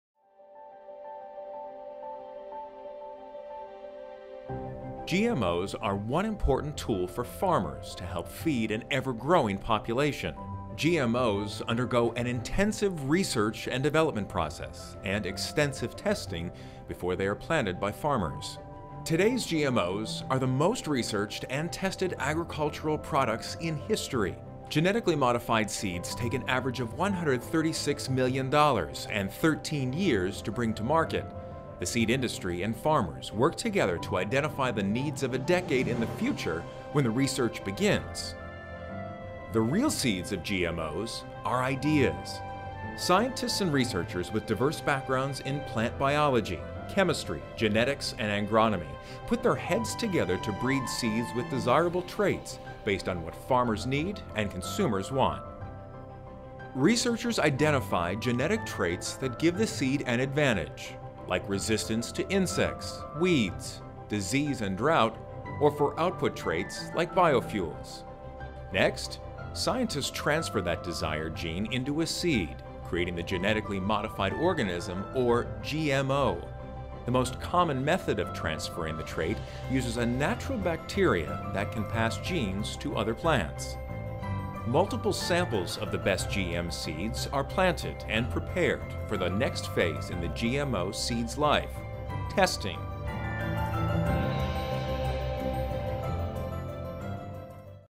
遺伝子組み換え作物誕生の舞台裏Ⅰ：コンセプト、研究、開発
遺伝子組み換え作物（GM作物）の生産には、厳格な手順が必要です。GM作物は歴史上最も研究され、試験が行われた農産物のひとつであることをご存知でしょうか？

このビデオでは、GM作物の種子が農場に植えられる前の初期のライフステージについて知ることができます。GM作物の開発プロセスには費用がかかり、完成までに何年もかかることがあります。農業や環境に最も有益な形質を特定するために、多くの専門家によって幅広い試験が行われます。

動画作成：GMO Answers
From concept to crop: how are GMOs made?